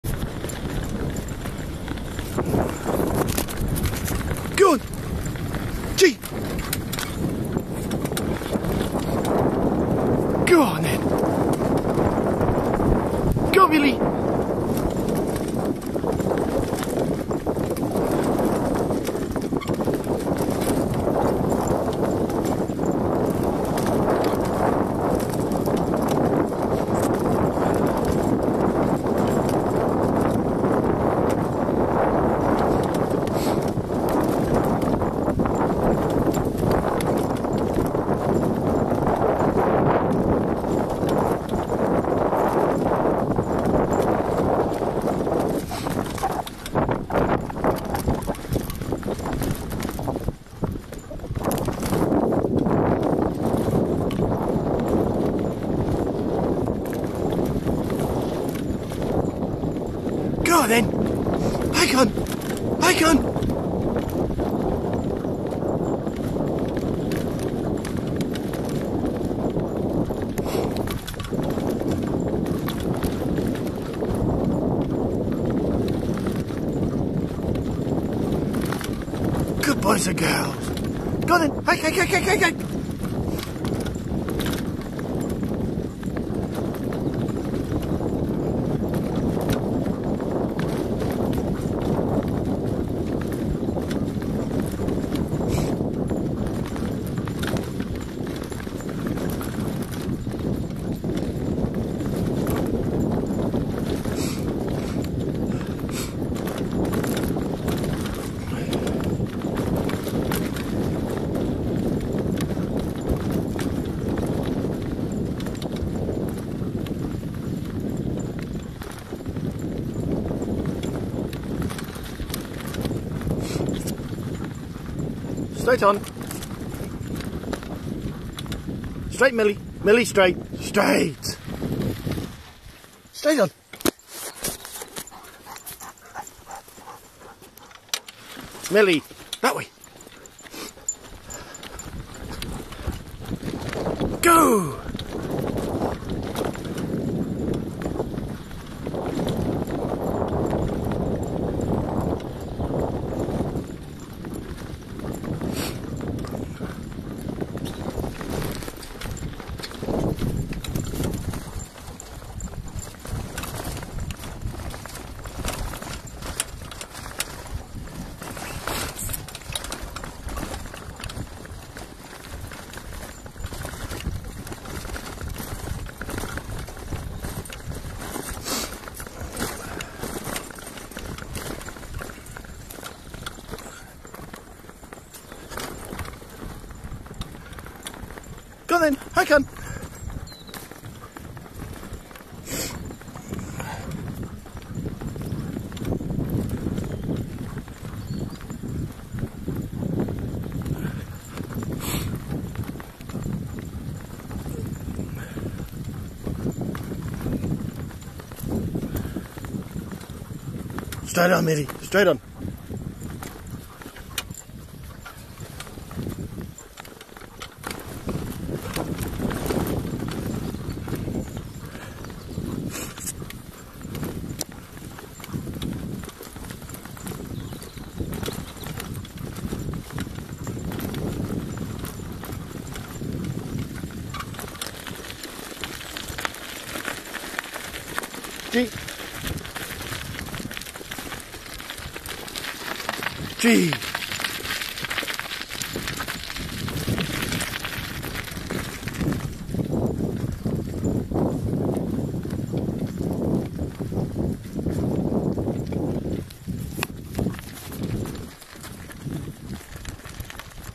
Malamutes in harness
mother and son working in harness